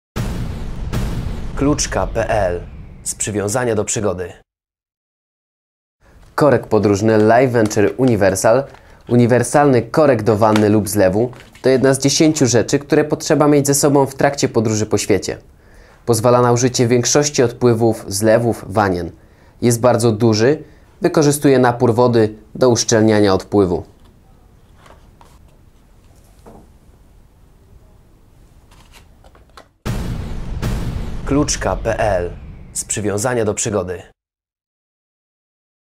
LIFEVENTURE - Korek podróżny - Universal - Kluczka.pl
Produkt dostępny na Kluczka.pl

Uniwersalny korek do wanny lub zlewu to jedna z 10 rzeczy, które trzeba zawsze mieć ze sobą w trakcie podróży po świecie. Pozwala na użycie w większości odpływów zlewów/wanien na całym świecie. Jest bardzo duży, wykorzystuje napór wody do uszczelnienia odpływu.

Wymiary: 120 x 120 x 5 mm
Waga: 40g